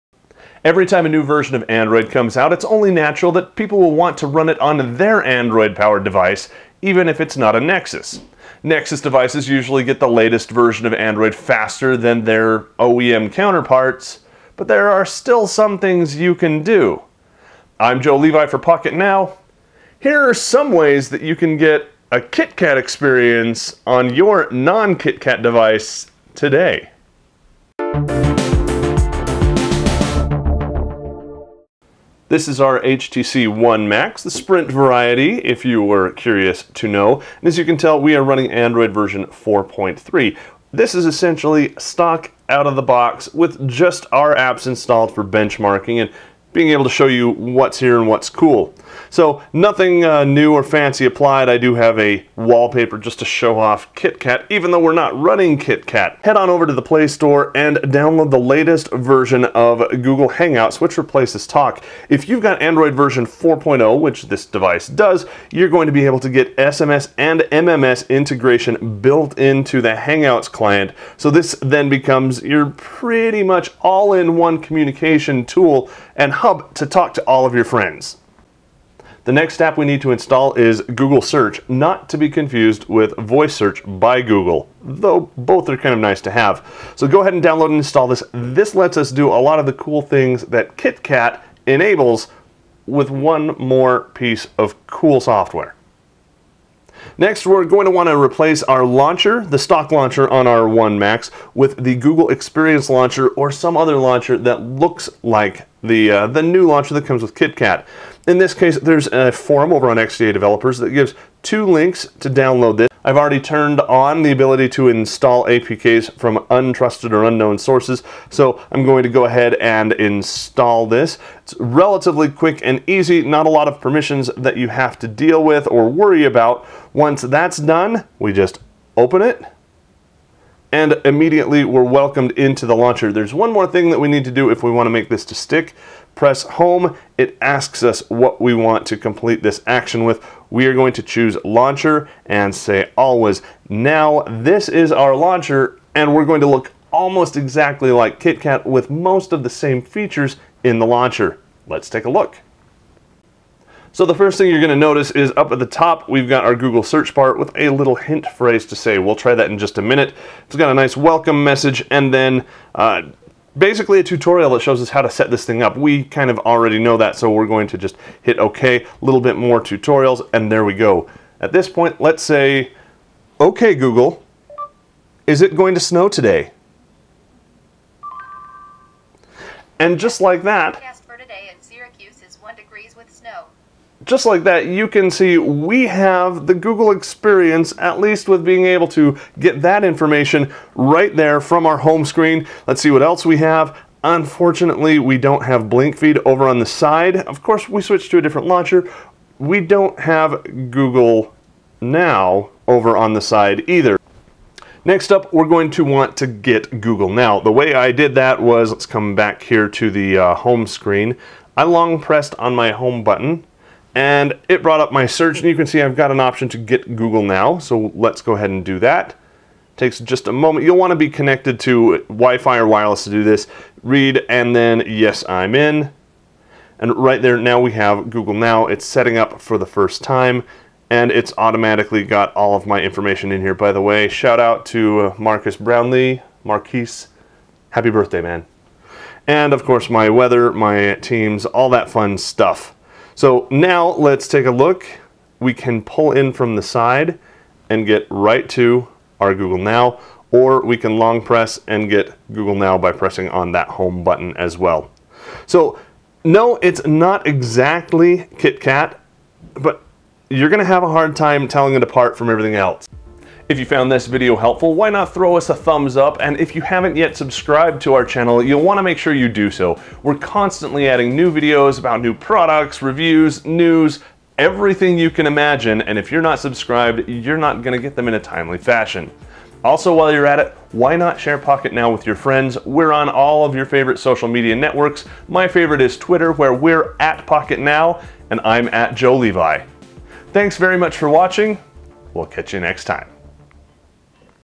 How to Get the Best Features of Android 4.4 KitKat on Your Older Phone | Pocketnow
Looking for links? /2013/12/03/best-features-of-kitkat

Every time a new version of Android comes out it's only natural that people will want to run it on their Android-powered device -- even if it's not a Nexus. Nexus devices usually get the latest version of Android faster than their OEM counterparts, but there are still some things that you can do to get a KitKat experience on your non-KitKat device today!